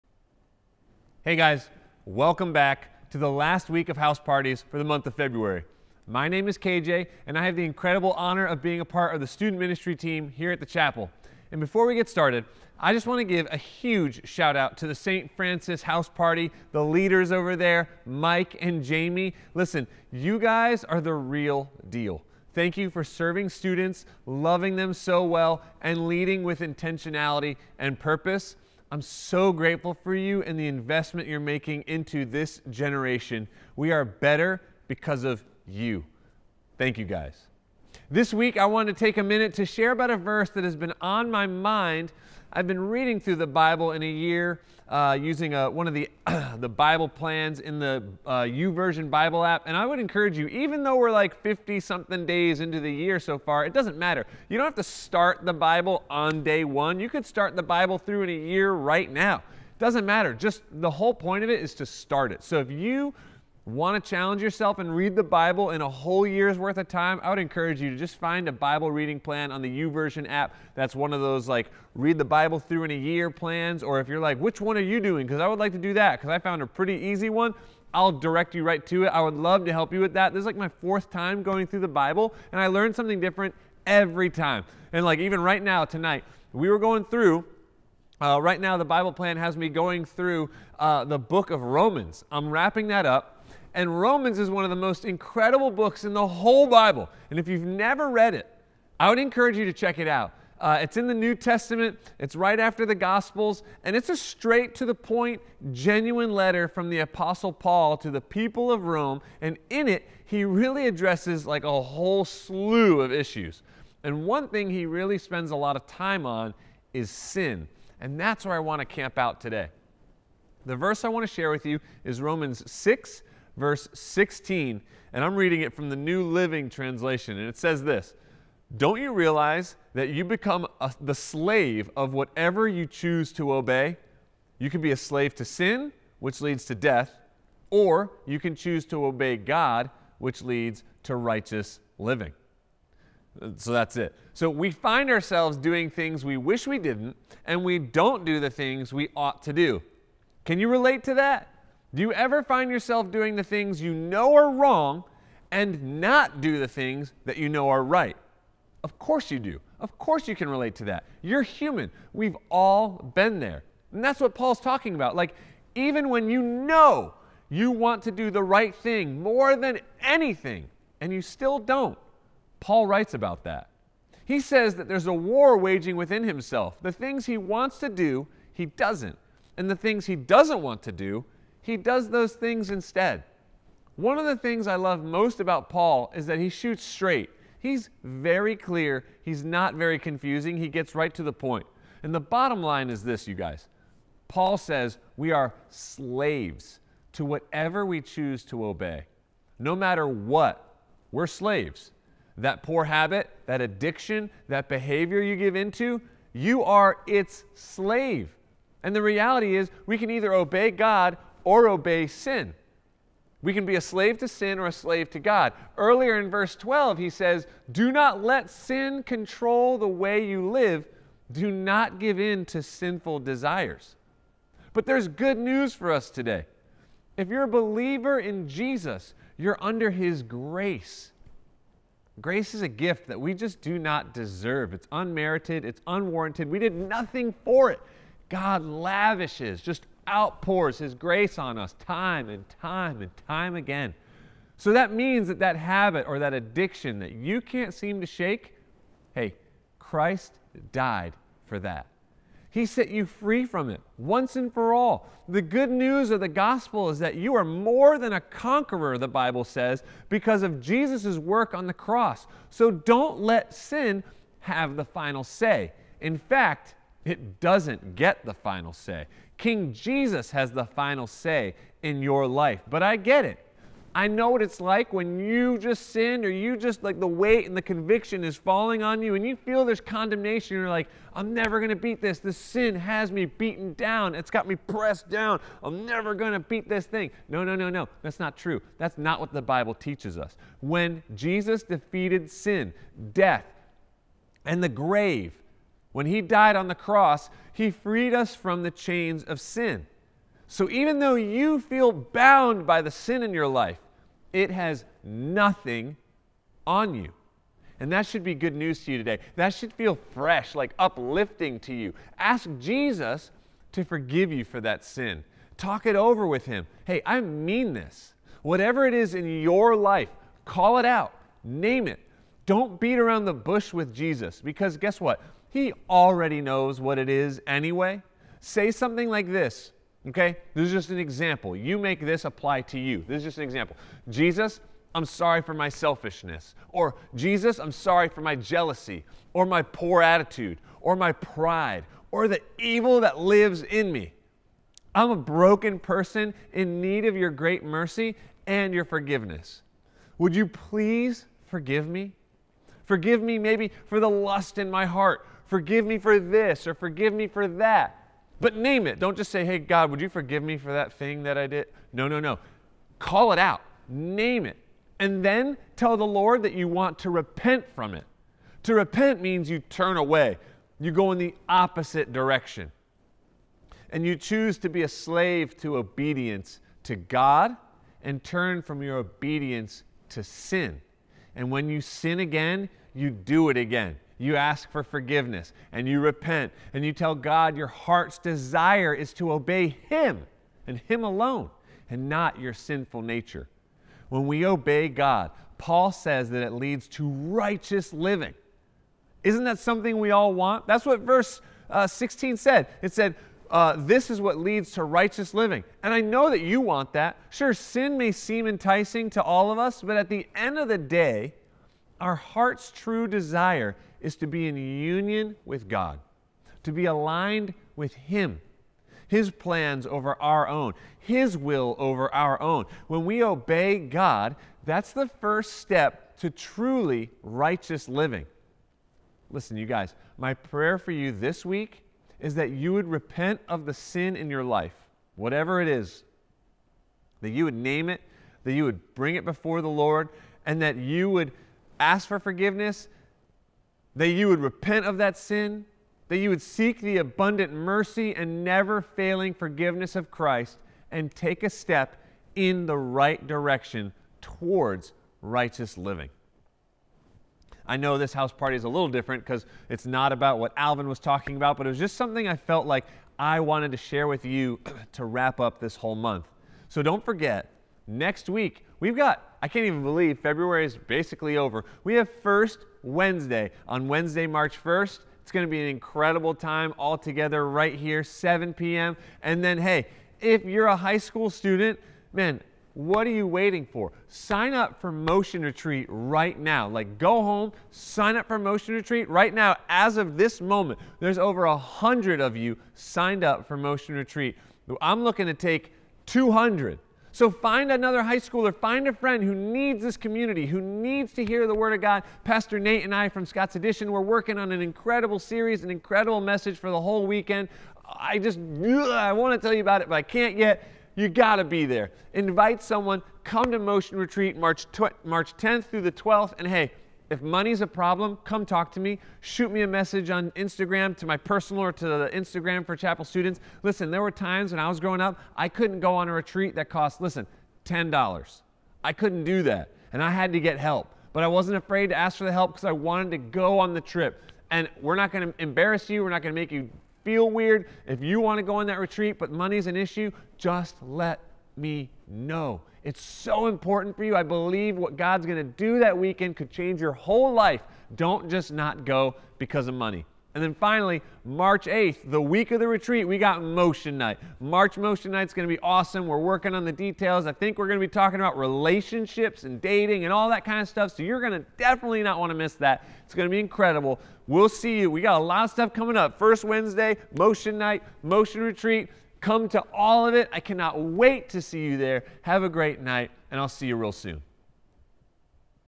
February House Party – Week 3
This week we spend a few minutes looking over Romans 6:16 (NLT), sin, and also how to seek forgiveness for it!